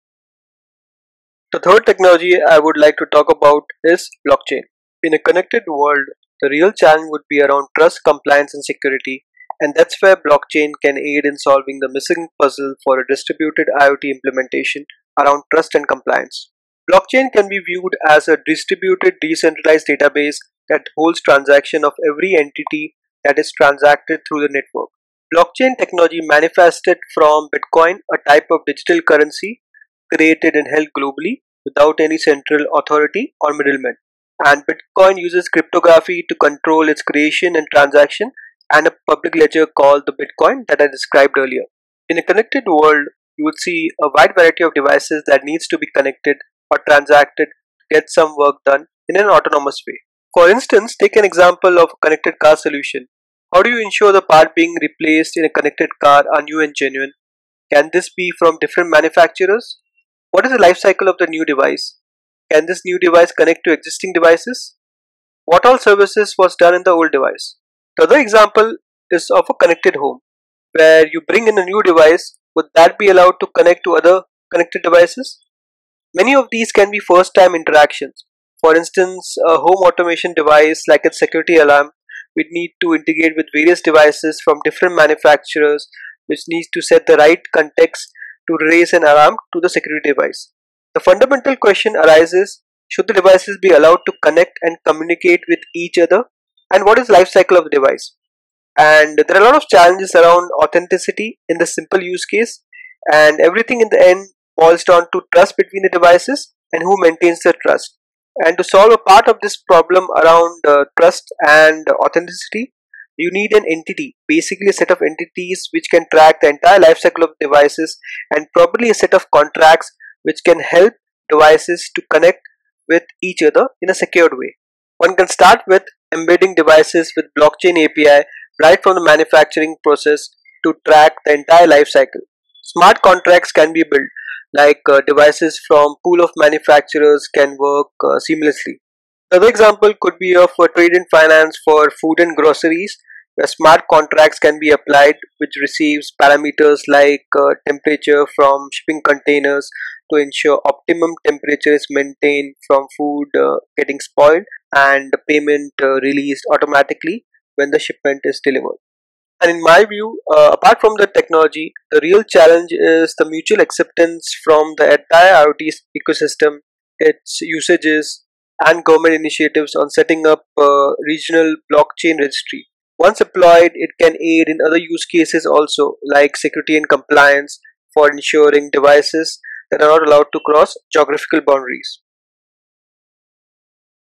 Blockchain for IoT & Artificial Intelligence
Understand Blockchain for IoT in 2 minutes. The combinatorial power of Blockchain, AI and IoT can help to realize the future of a smart connected world.